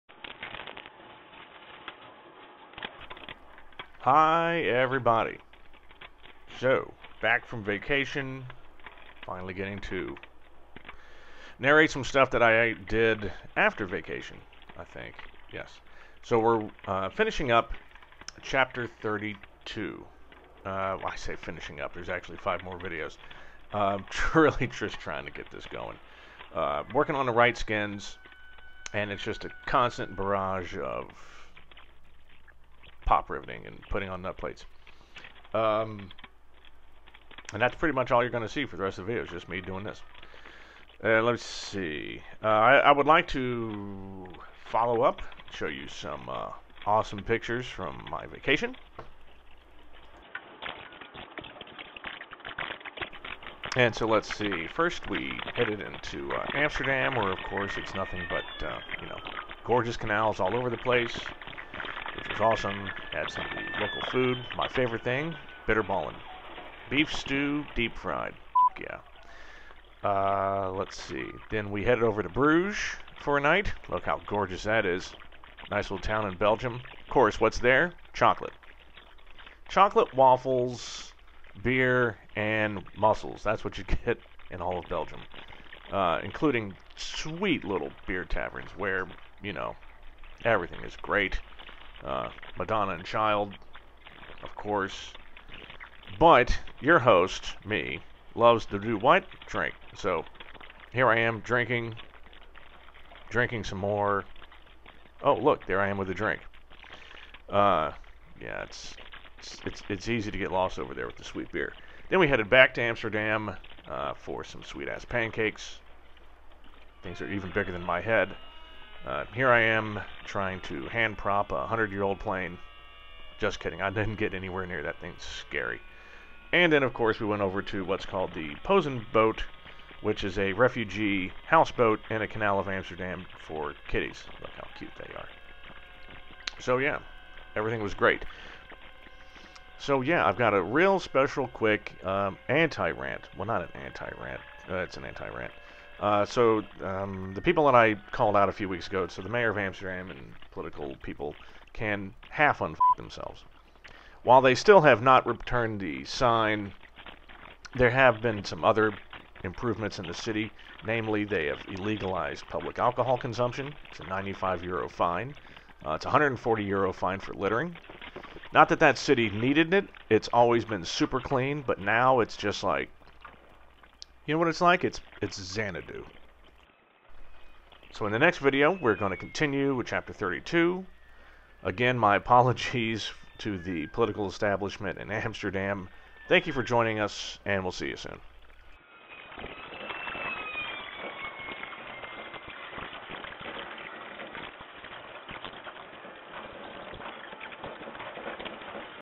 RV - 14 Ch 32 - 06
First work back from vacation. A few pics from the old country. Apparently it was 'sweet'. Plus a rare thing... an anti rant. Its not a retraction :D